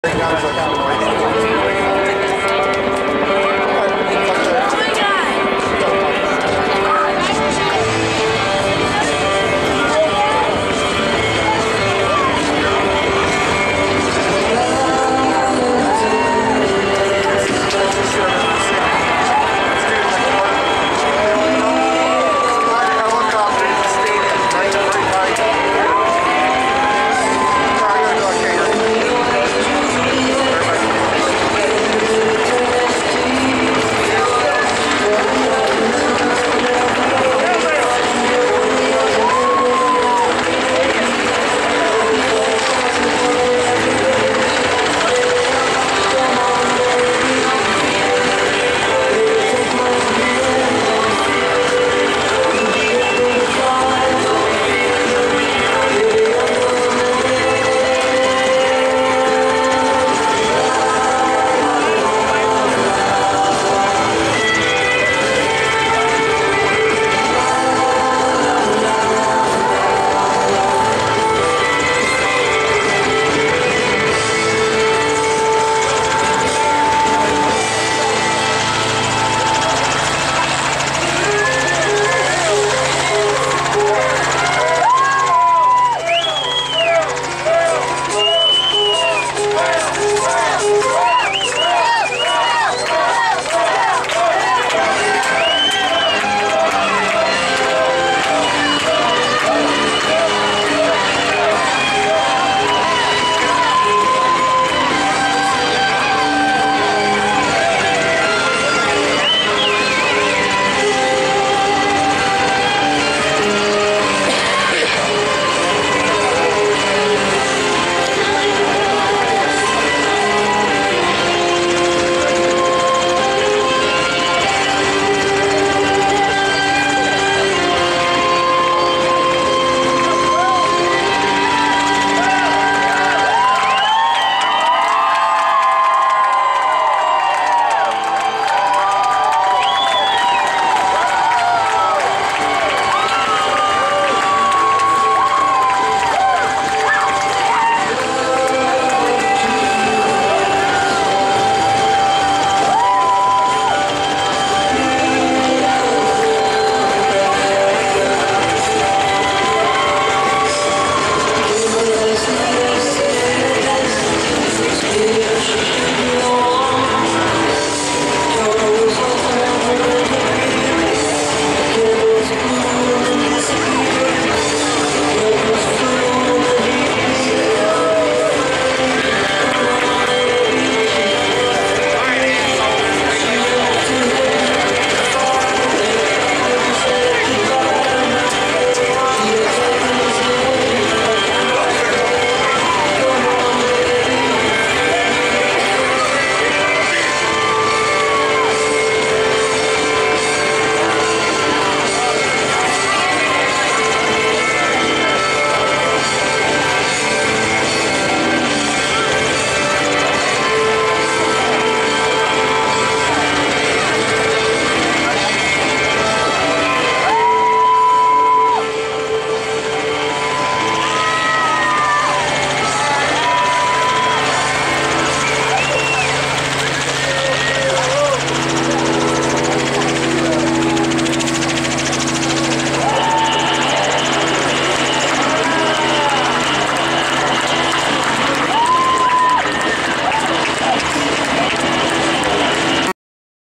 Will Ferrell arrives at Camelback Ranch Stadium (raw footage)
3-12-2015:  Will arrives to play DH for the Chicago White Sox & then catcher for the San Francisco Giants.